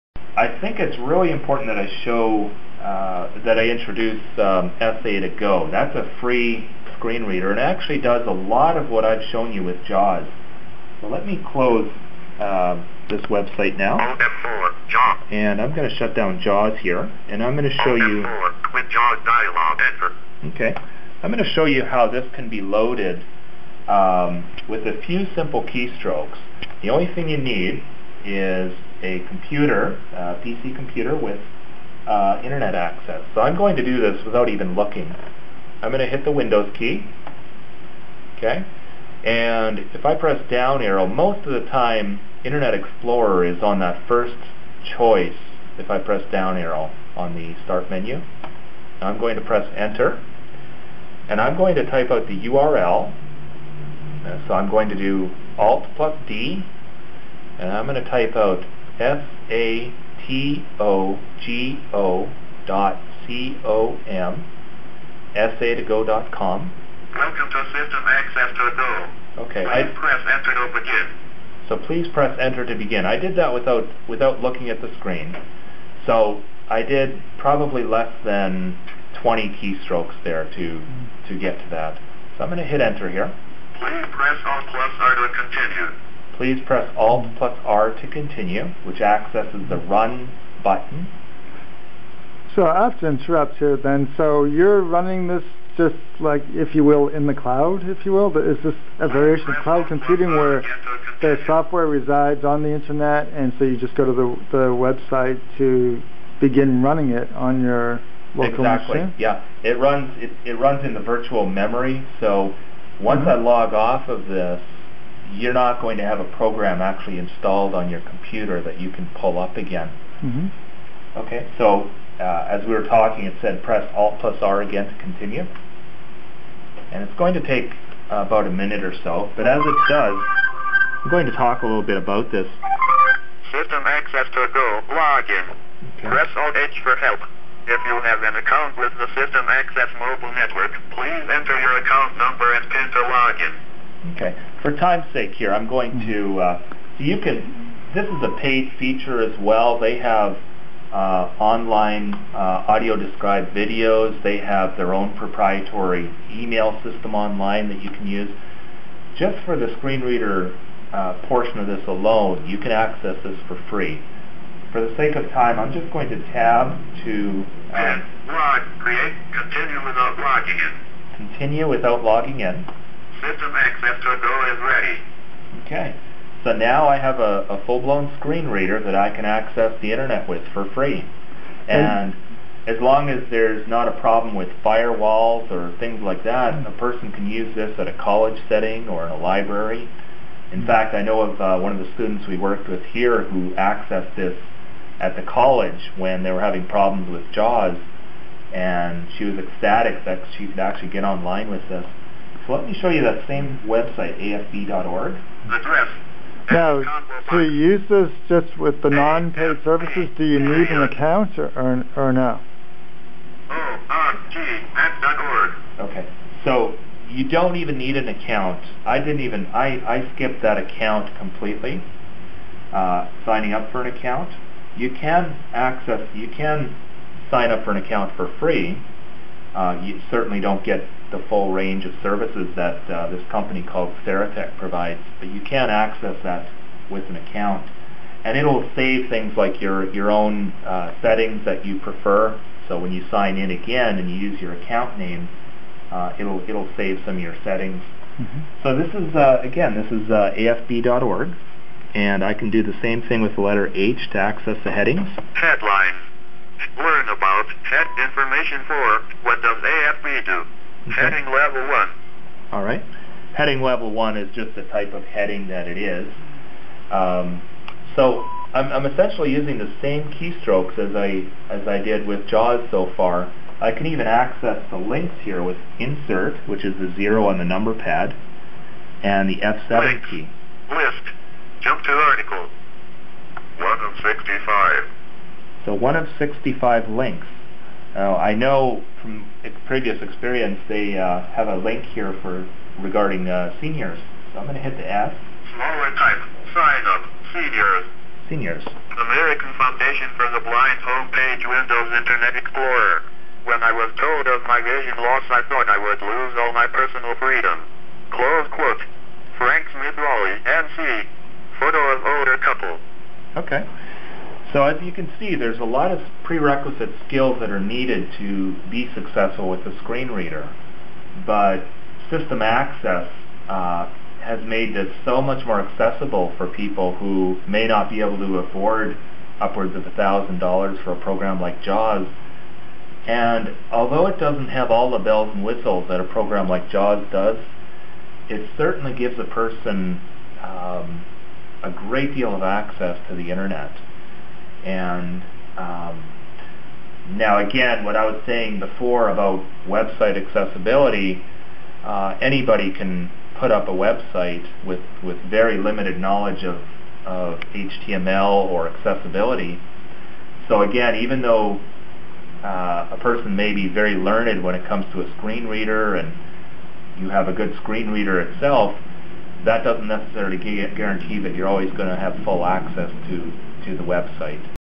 Basic overview of the screen reader System Access
Basic overview of the System Access screen reader distributed by Serotek, including the steps to load it and basic shortcut keystrokes to navigate a web page.